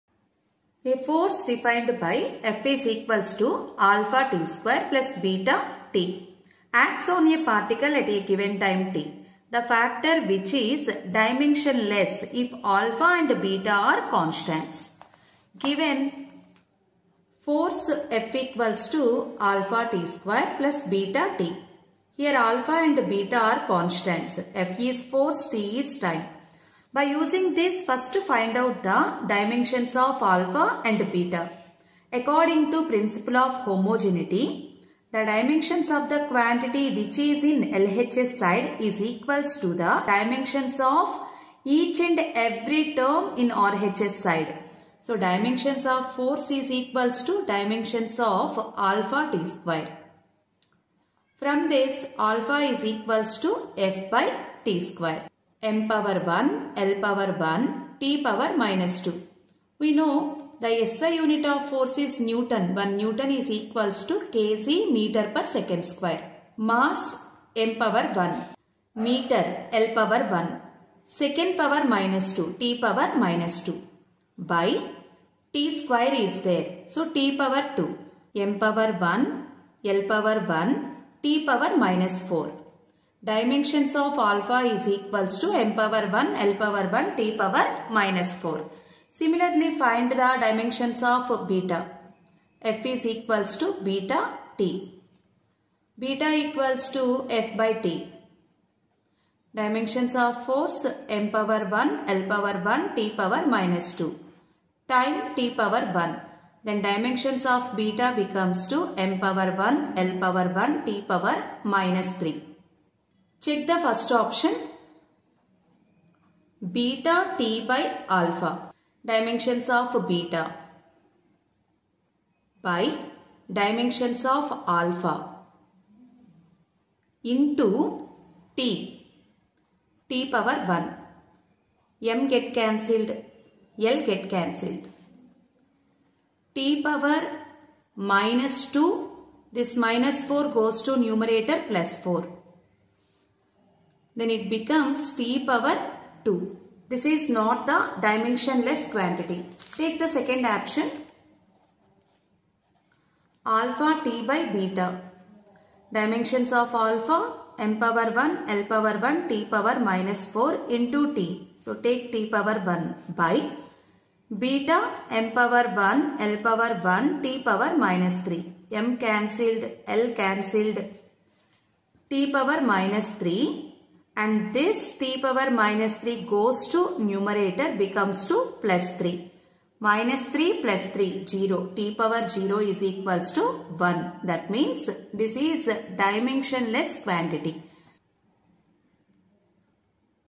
A force defined by F=alphat^2+betat acts on a particle at the given time t.The factor which is
A force defined by F=alphat^2+betat acts on a particle at the given time t.The factor which is dimensionless if alpha and beta are constants,is
neet physics pyqs detailed explanation
neet pyqs from units and measurements